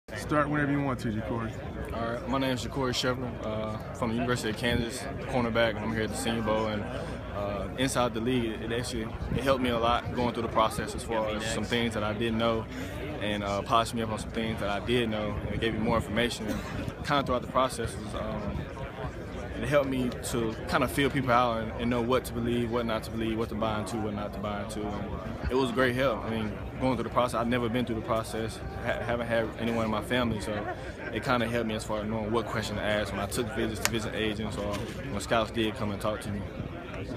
Kansas' Jacorey Shepherd on the ITL Newsletter
From the Senior Bowl in Mobile, Ala. in Jan. '15, Jacorey Shepherd gives his thoughts on the free newsletter describing the draft process, provided by ITL .